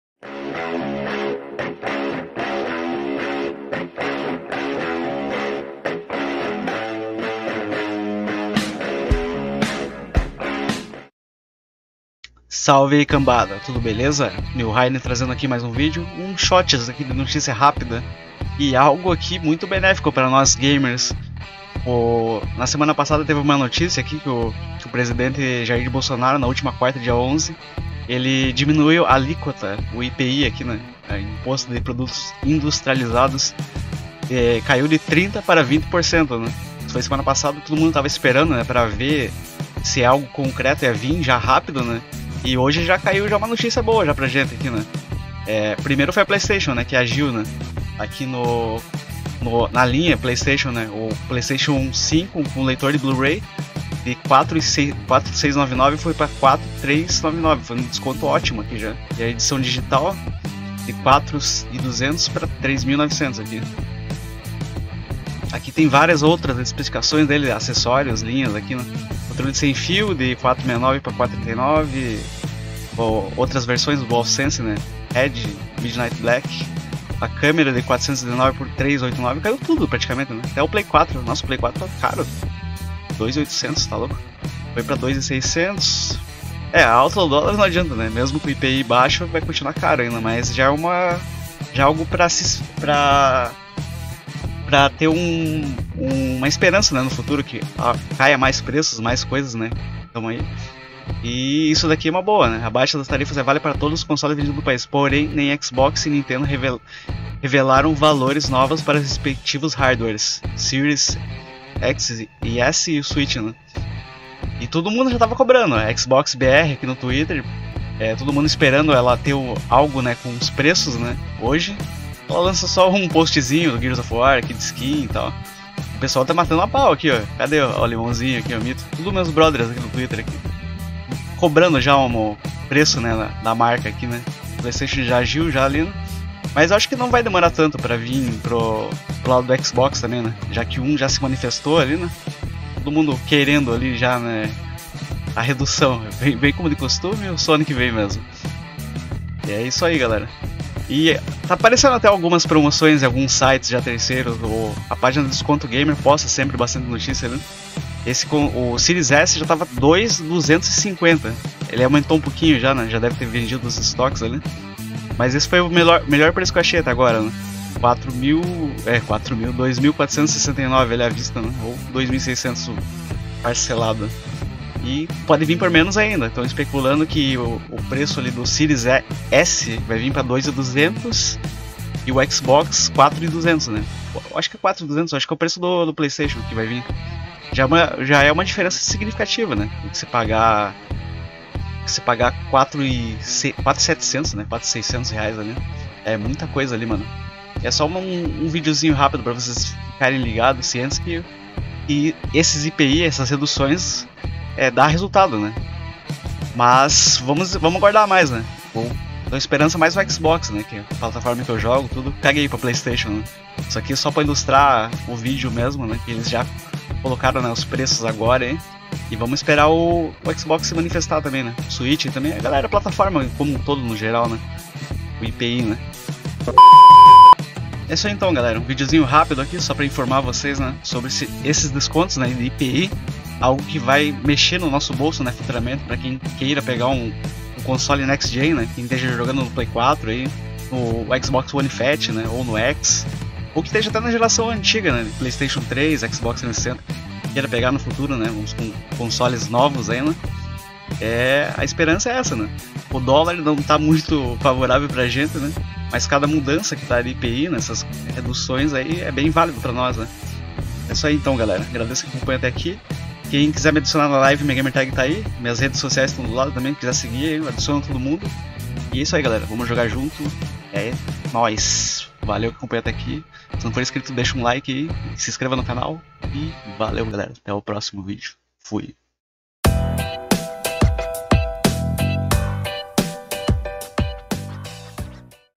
REDUÇÃO de IMPOSTO aprovada e já em Prática! Playstation 5 e Acessórios mais Baratos!
Fala Galera, novidade muito boa para nós fãs de Games
Governo anuncia nova redução do IPI para jogos eletrônicos, as alíquotas do IPI caem de 30% para 20%
Jair Bolsonaro, na última quarta-feira (11), para incentivar o desenvolvimento do segmento no país. Esta é a terceira redução do imposto para o setor em dois anos.
No dia de hoje já tivemos uma redução significativa em toda a linha de console e acessórios da SONY Playstation
estamos aguardando a Microsoft BR se manifestar e dar o preço sugerido com redução em Reais

Links nos comentários das matérias (;

PSN ID: xNill Ray
Gamertag: x NILL RAYDEN x